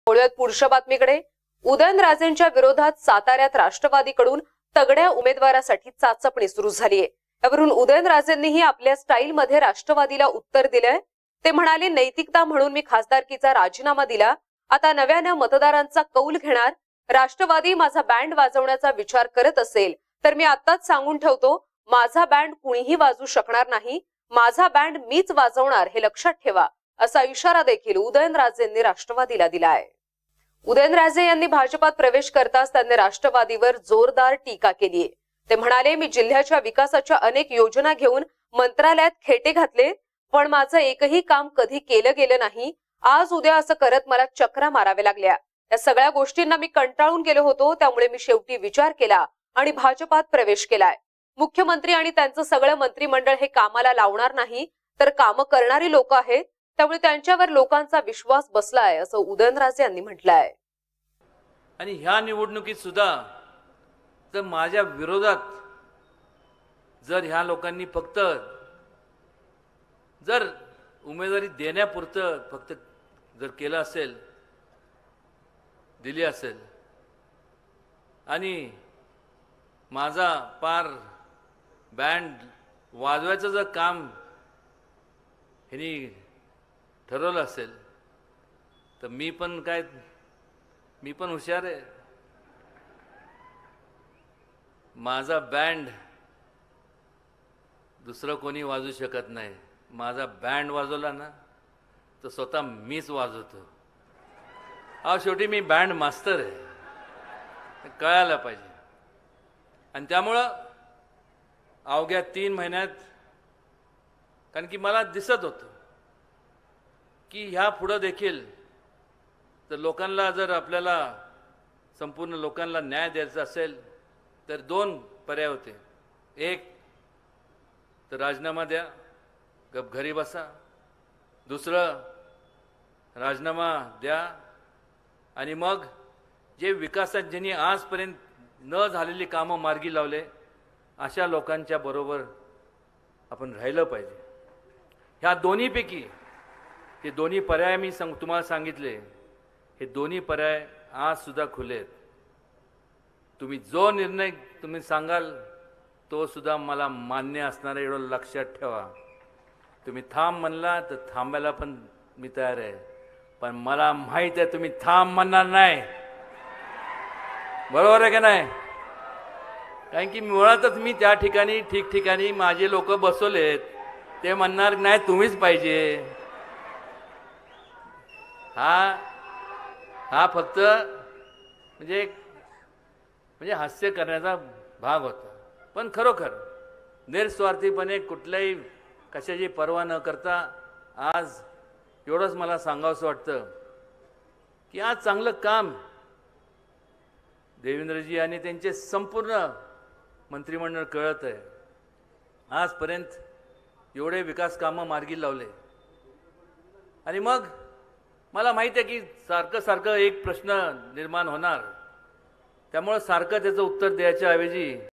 सातारा माझा बँड मीच वाजवणार हे लक्षात ठेवा उदयनराजेंच्या राष्ट्रवादीला इशारा
माझा बँड मीच वाजवणार हे लक्षात ठेवा